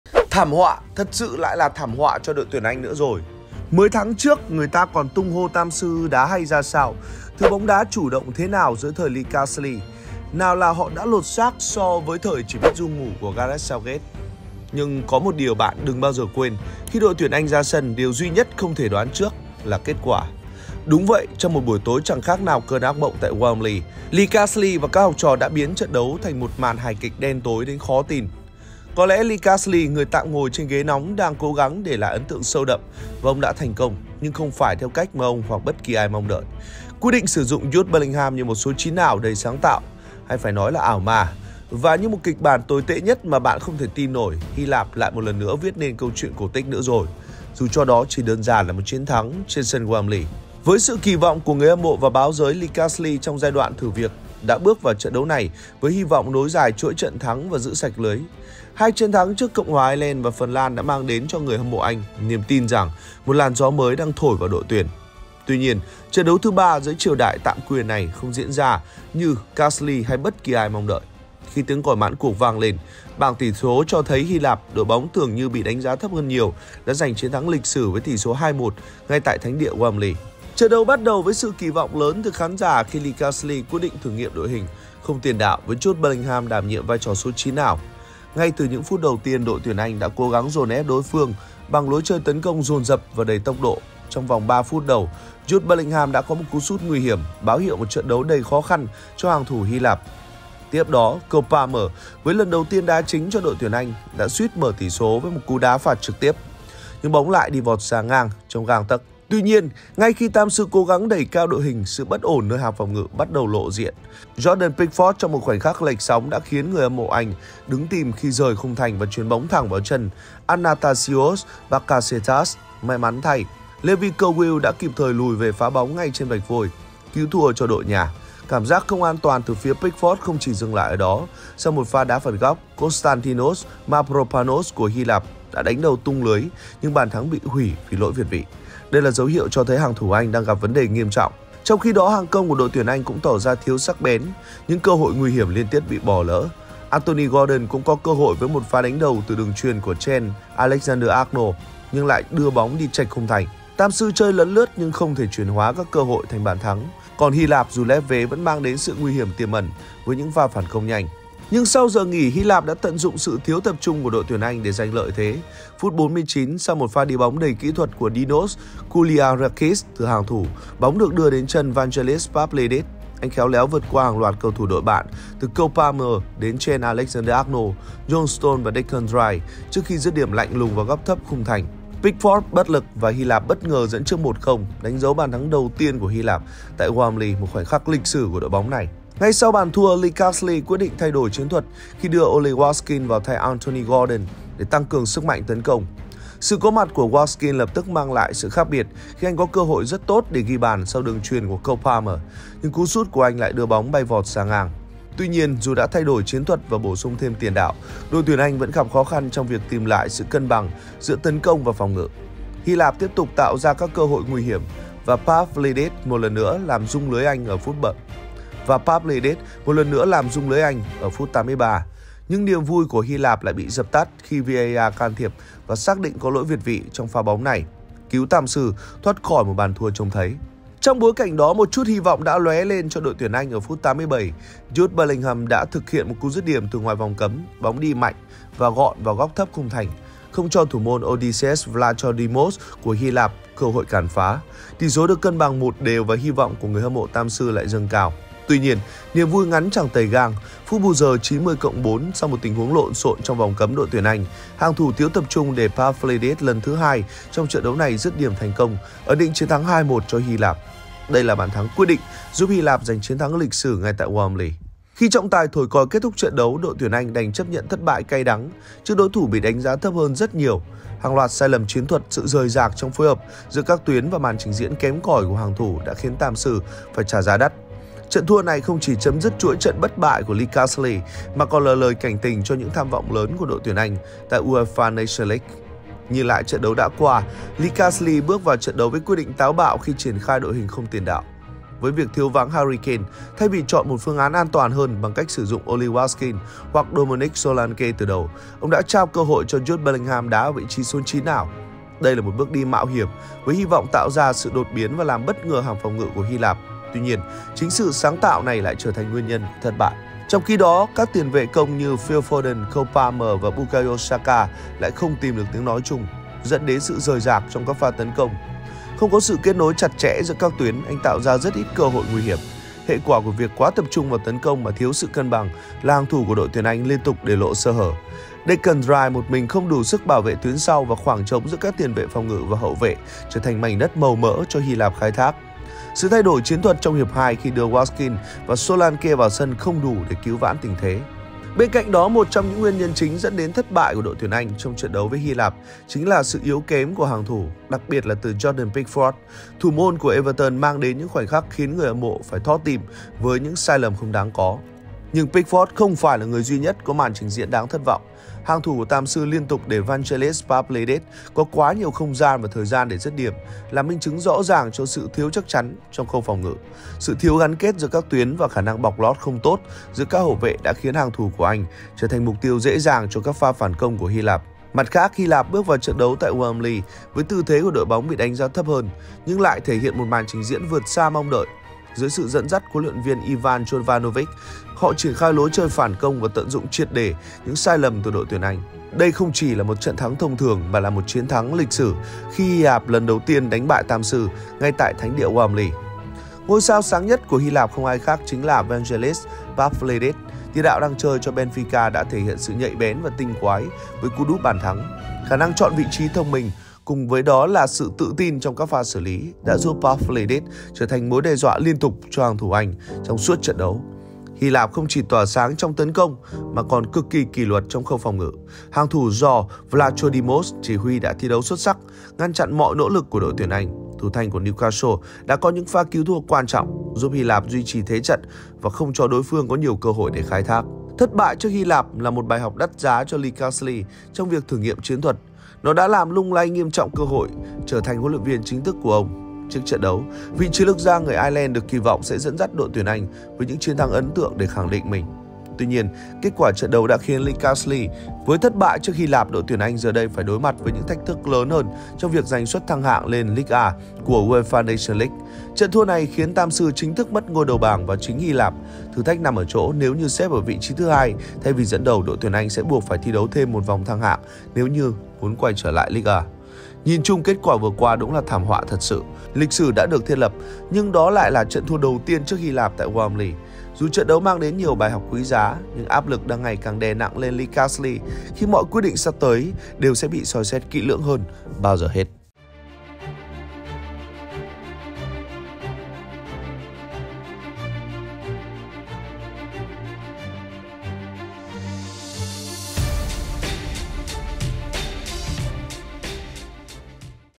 THUA SỐC TRƯỚC HY LẠP TRÊN SÂN NHÀ, THẢM HỌA ĐỘI TUYỂN ANH
Gareth Southgate.
Thảm họa, thật sự lại là thảm họa cho đội tuyển Anh nữa rồi. Mới tháng trước người ta còn tung hô Tam Sư đá hay ra sao, thứ bóng đá chủ động thế nào dưới thời Lee Carsley. Nào là họ đã lột xác so với thời chỉ biết ru ngủ của Gareth Southgate.

Nhưng có một điều bạn đừng bao giờ quên, khi đội tuyển Anh ra sân, điều duy nhất không thể đoán trước là... kết quả. Đúng vậy, trong một buổi tối chẳng khác nào cơn ác mộng tại Wembley, Lee Carsley và các học trò đã biến trận đấu thành một màn hài kịch đen tối đến khó tin.